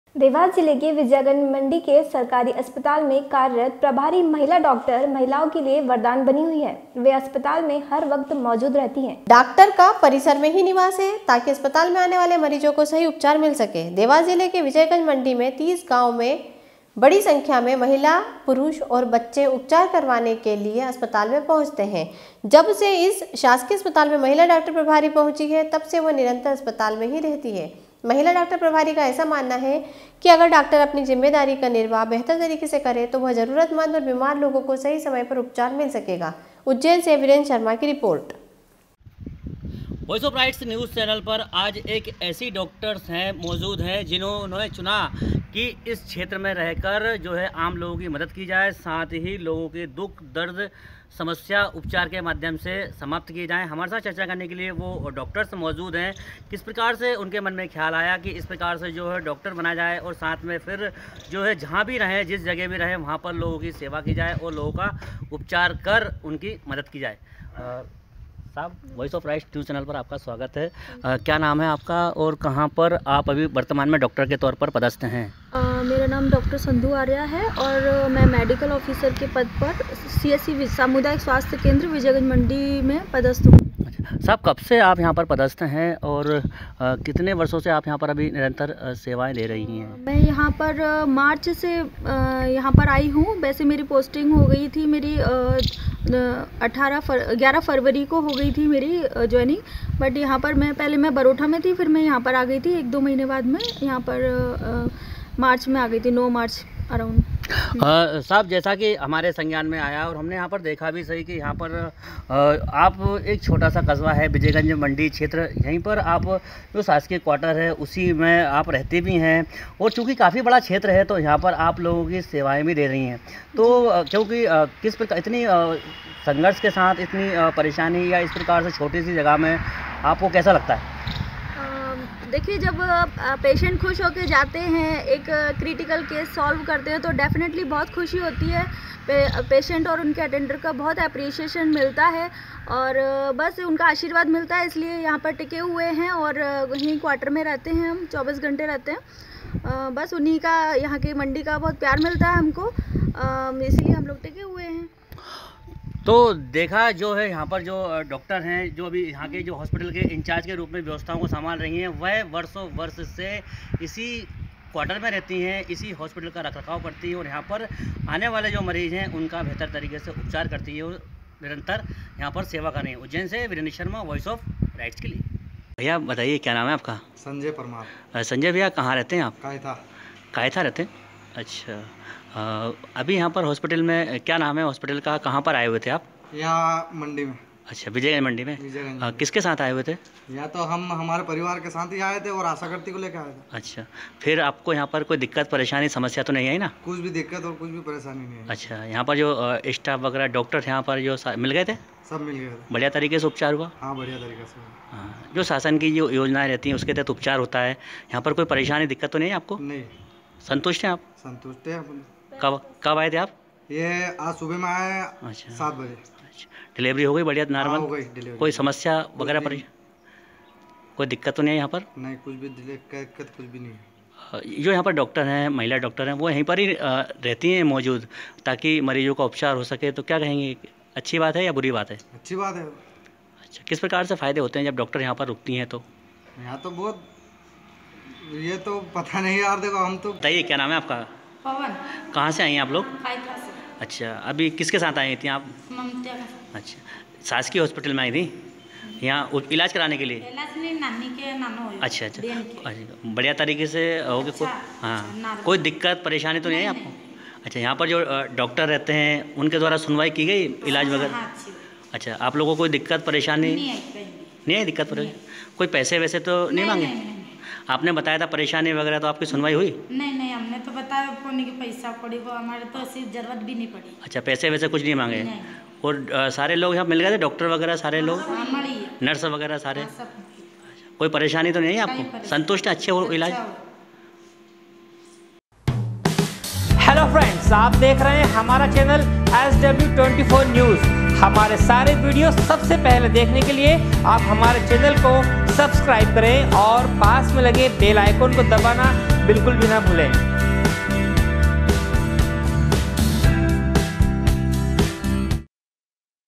Dewas- सरकारी अस्पताल में कार्यरत प्रभारी महिला डॉक्टर महिलाओं के लिए वरदान बनी हुई है।
Dewas- SW 24 News Madhyapradesh
देवास जिले के विजयगंज मंडी के सरकारी अस्पताल में कार्यरत प्रभारी महिला डॉक्टर महिलाओं के लिए वरदान बनी हुई है। वे अस्पताल में हर समय मौजूद रहती है। उज्जैन से वीरेंद्र शर्मा की रिपोर्ट